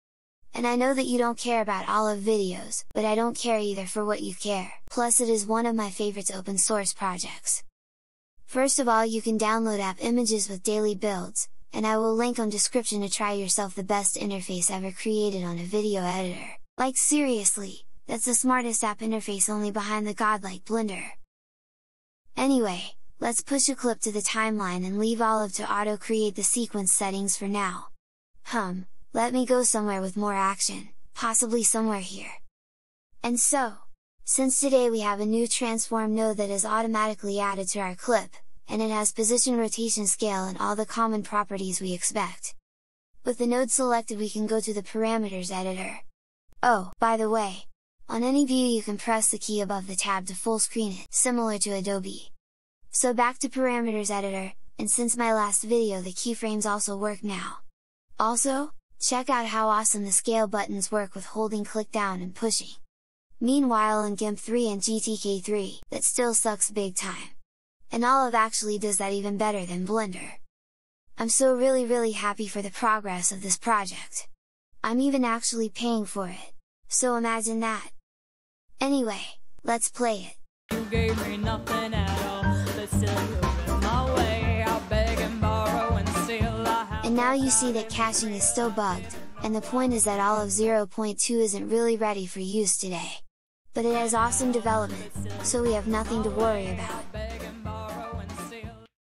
Olive 0.2 today added a Transform Node \o/
updates to my favorite video editor, and my most used hobbyist app!!